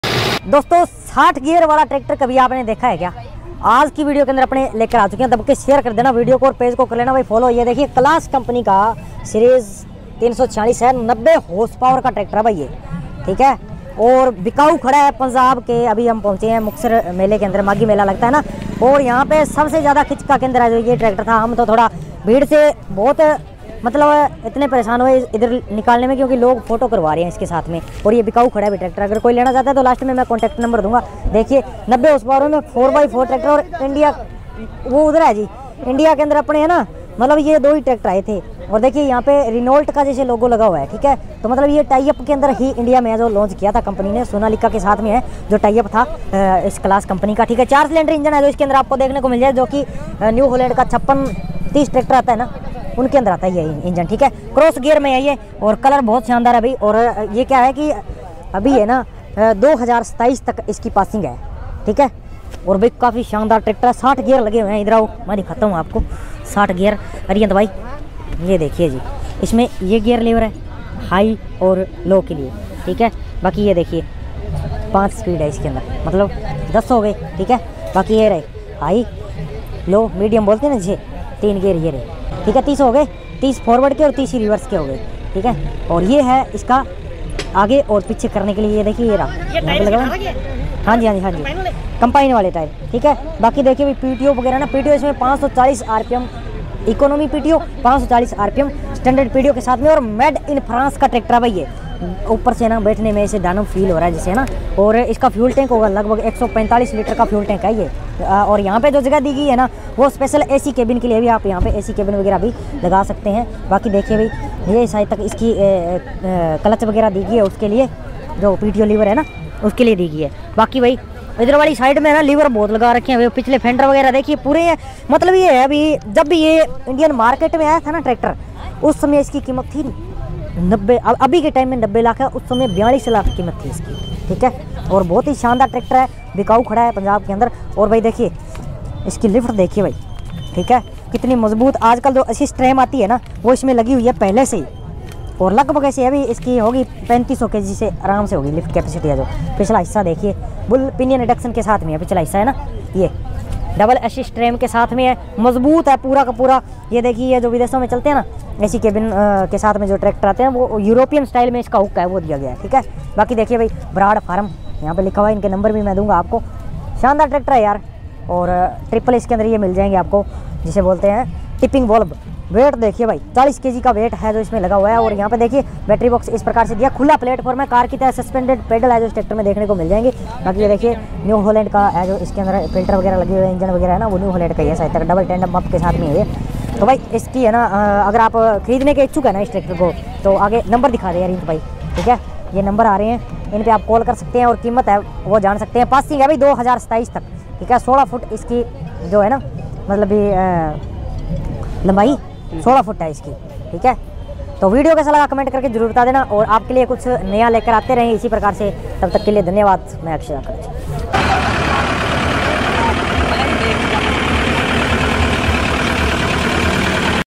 भारत में आया 60 गियर वाला क्लास ट्रैक्टर Claas tractor 4✖️4 Made in Japan 🇯🇵
Power king tractor 90hp भारत में आया 60 गियर वाला क्लास ट्रैक्टर Claas tractor 4✖️4 Made in Japan 🇯🇵